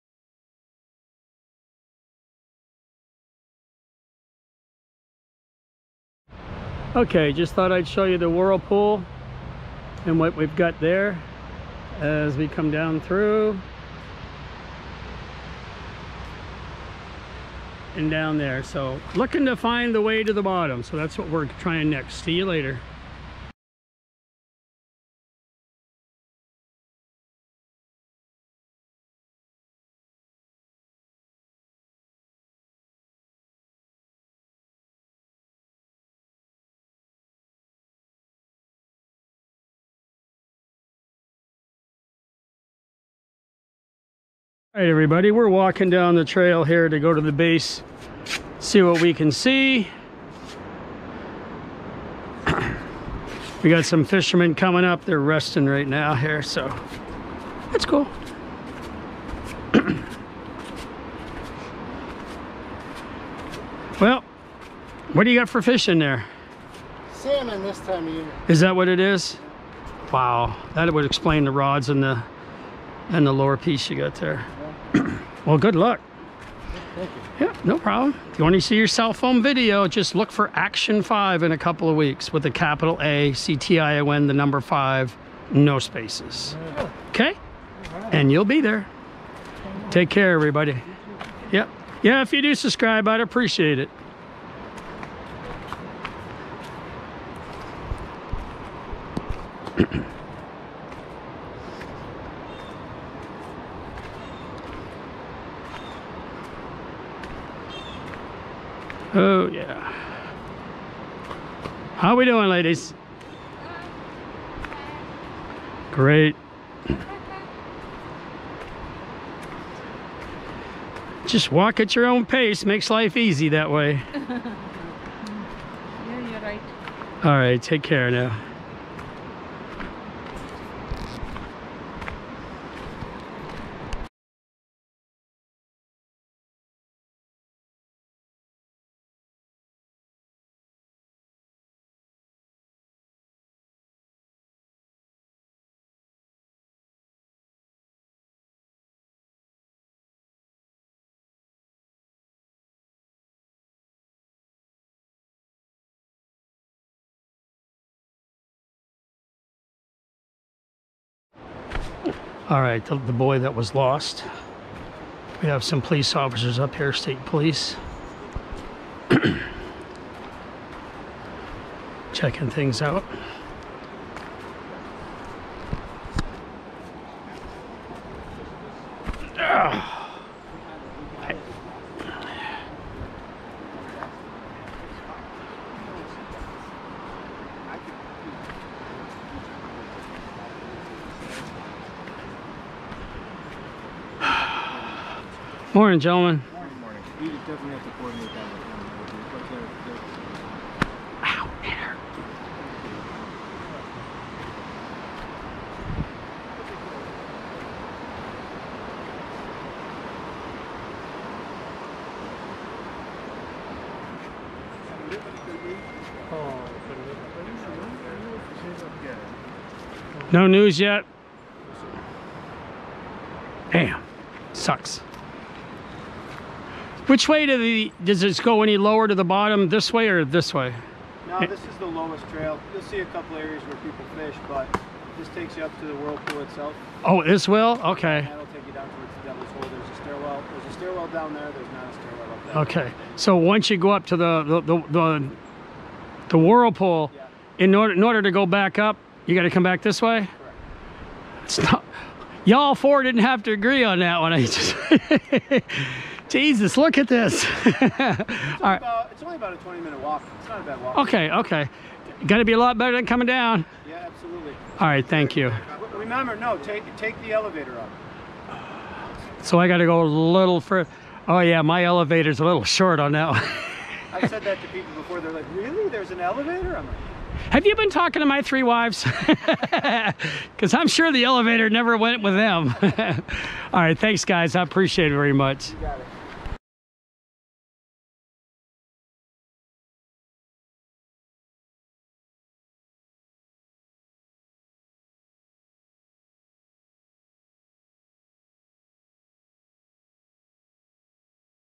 Last stop the Whirlpool not to exciting but its a great hike down to the bottom. The END
This video will cover the walk down to the river. From the parking lot its and easy walk. to the trail . Then stone walk way all the way down. Free parking in the lot. its not all that exciting after you done the boat and the cave but it is different.